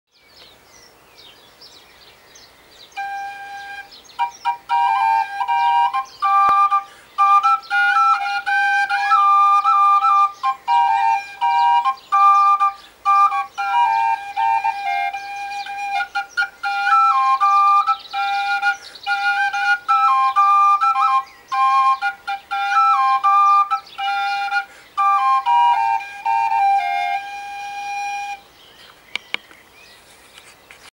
Cantiga de Santa Maria on a one-handed drone pipe.
A demo of the pipe developed as a reconstruction of a carving on Beverley Minster. I am not even attempting to play te tabor with it, as I never have done so.